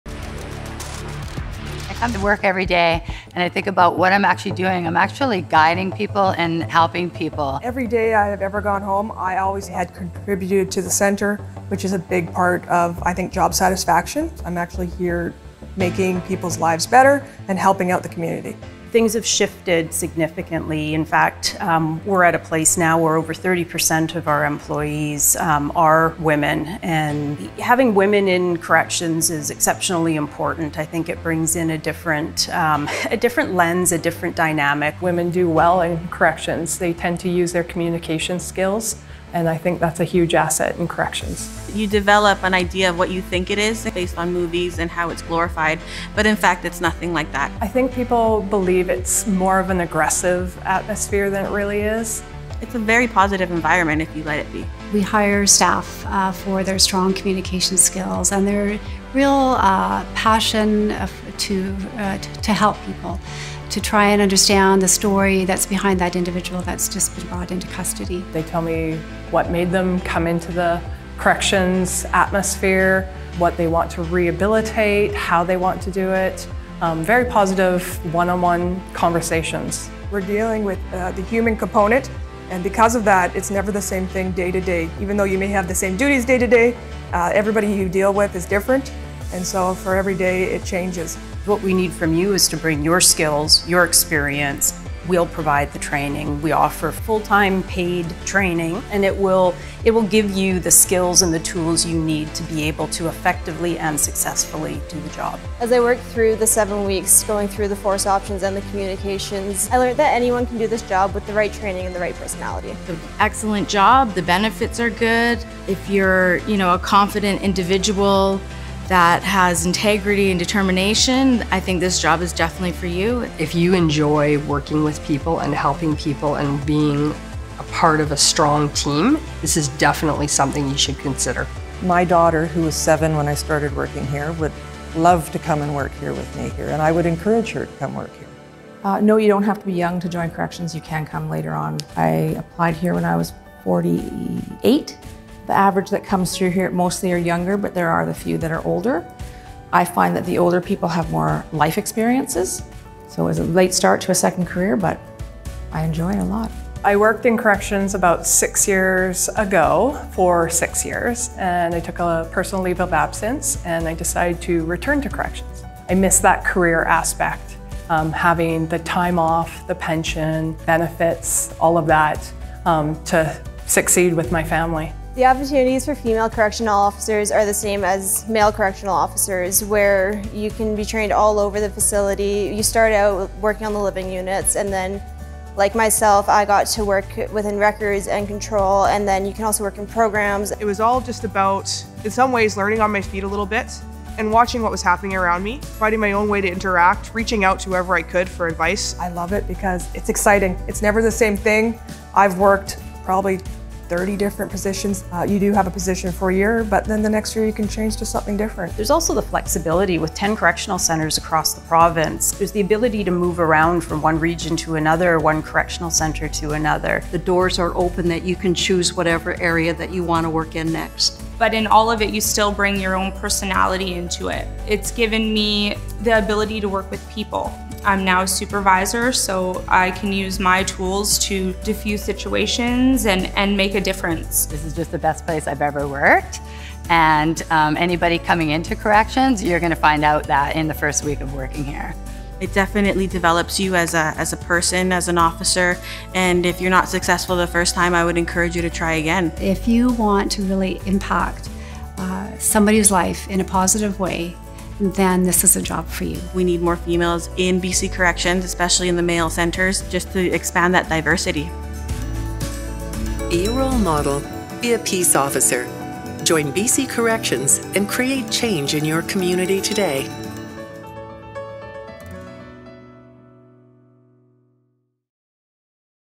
Women in BC Corrections
The number of women working in corrections has been steadily increasing over the years and this job is not about being a physically dominant figure – it’s about compassion. People think in order to be a correctional officer you have to be really big and physically strong individual but that’s just a myth so forget the stereotype, this job is about communication, problem solving, de-escalation and teamwork. It’s a great career for women and men looking to make a difference in someone’s life. With the training provided by BC Corrections anyone can do this job and you will have the tools to make a positive difference in someone’s life. for more information or contact a recruiter by phone at or email at to book an interview preparation session.